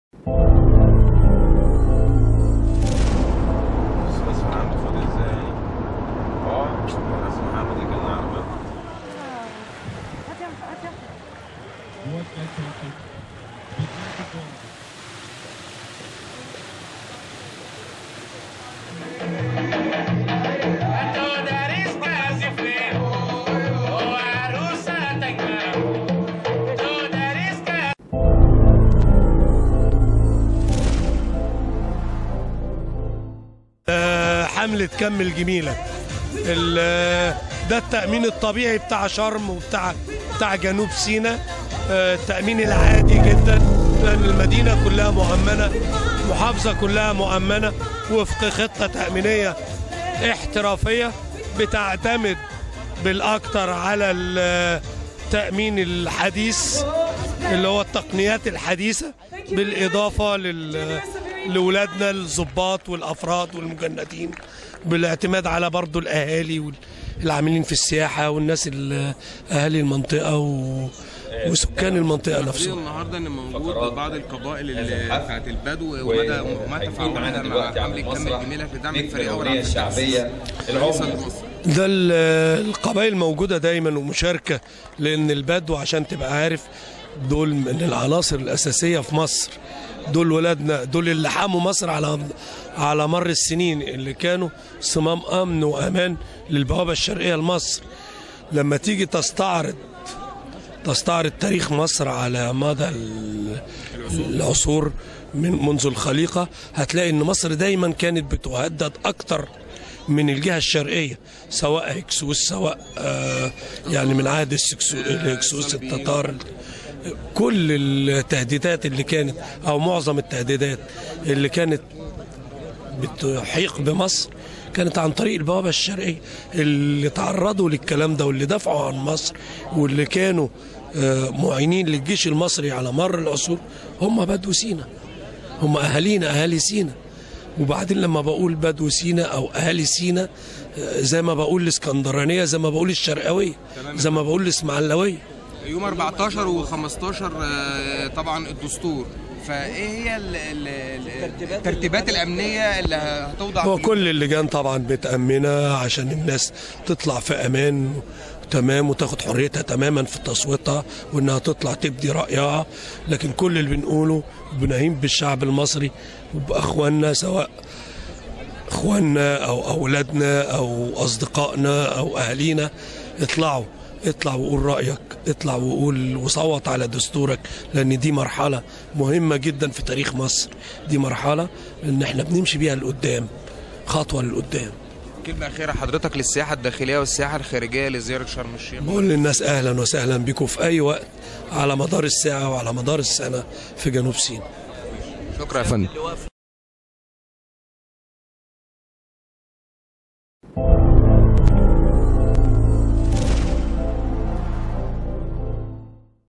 "مدير أمن جنوب سيناء" يدعو المواطنين للمشاركة فى الإستفتاء على الدستور
مدير أمن جنوب سيناء شرم أمنه تماماً
شرم الشيخ / محمد فاروق - أحمد رجب